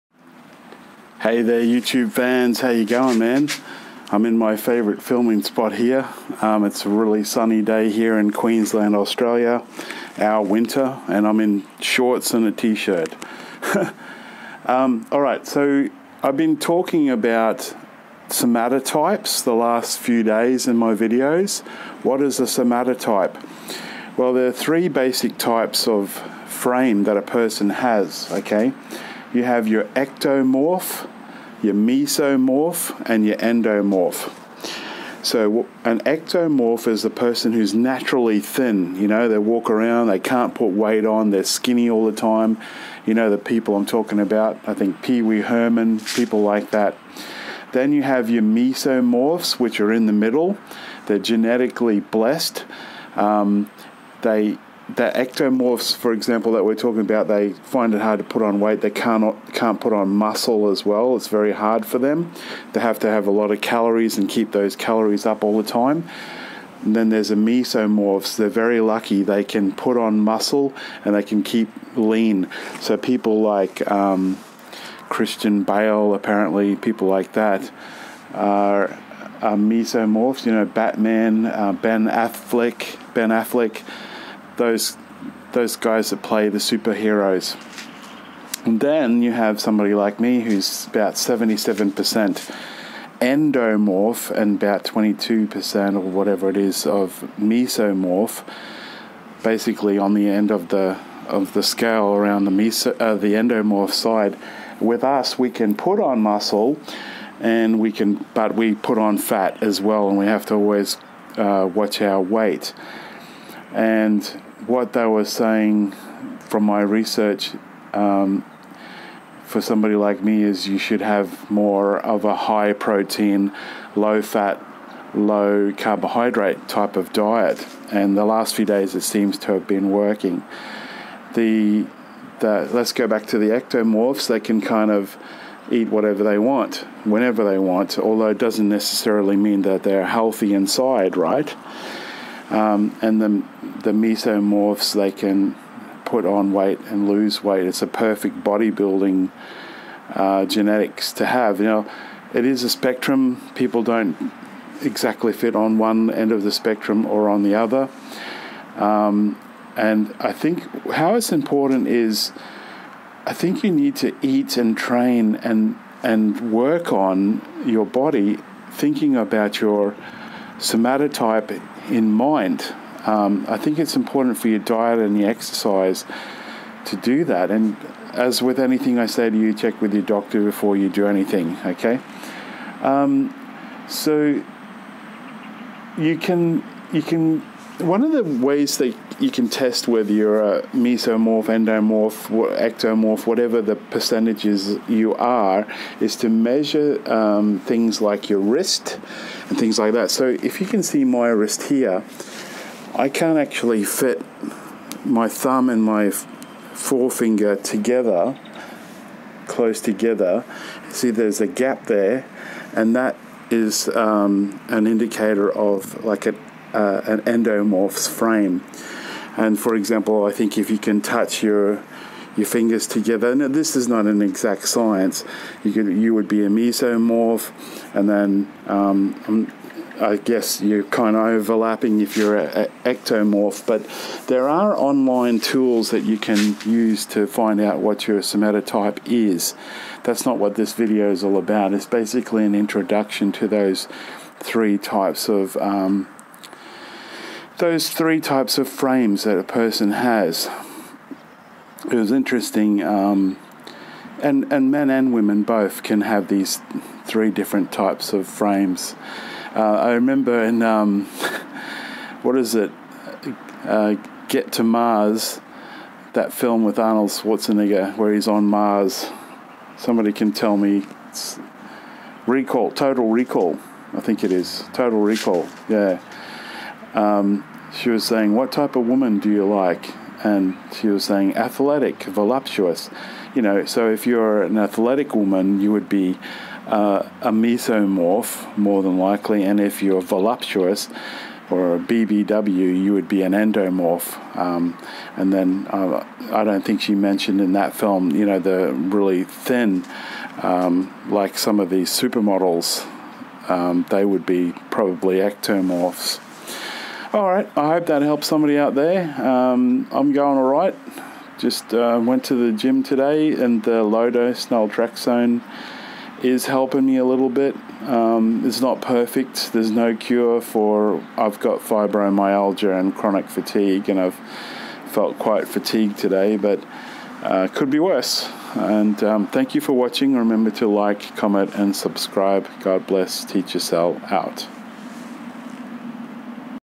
Your body type (Somatotypes). Ectomorph, Mesomorph, Endomorph
How to identify your body types in how to eat for your body type.
Ectomorph, Mesomorph, Endomorph